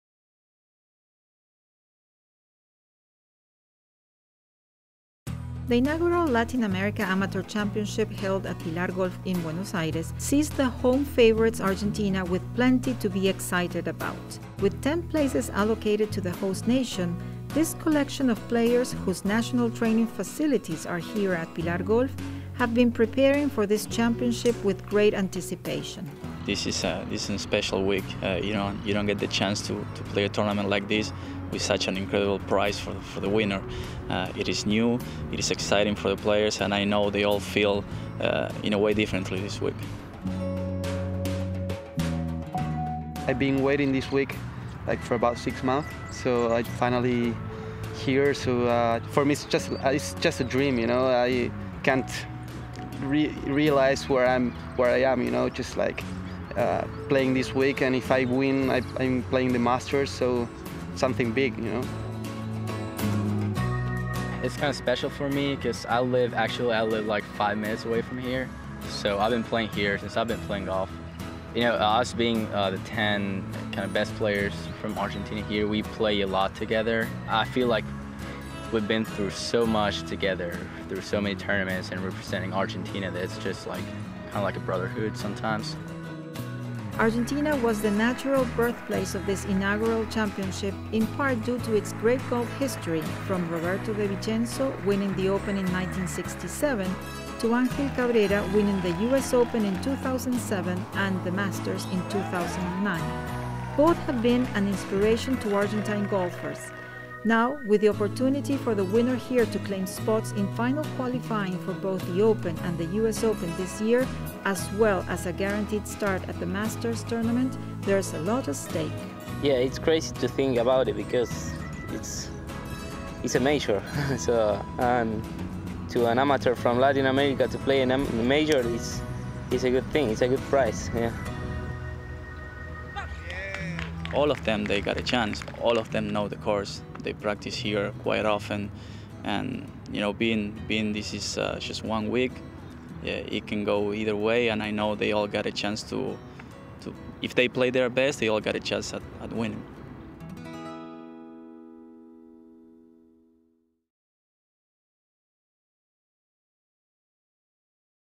GW Inside The Game: Latin America Amateur Championship - Team Argentina
We speak the the Argentinian contingent who lined up at the Latin America Amateur Championship in search of a place in the field at the Masters, and in final qualifying for The Open Championship and the U.S. Open